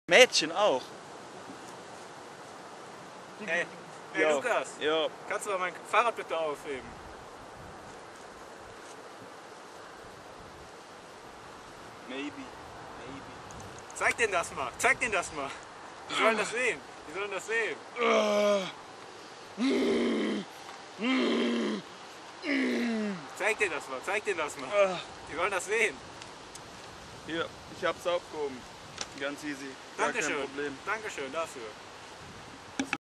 Zeig den DAS mal!! Ep 1 Pick up my Bike/Heb mal mein Fahrrad auf BITTE!!
Wicked man Guerilla at it´s finest!!!... When sombody asks yuh to pick up their bike and yuh Guerilla enough to overshoot the Goal!!!...

Wenn du gefrat wirst, ob du das Fahrrad aufheben kannst und Guerilla genug bist, über diese Bitte hinaus zu schießen!!...
(Guerilla Deutsch)

Big up for my Guerilla pan di Cam MysteriousJC
Check his Channel: ZEIG DEN DAS!...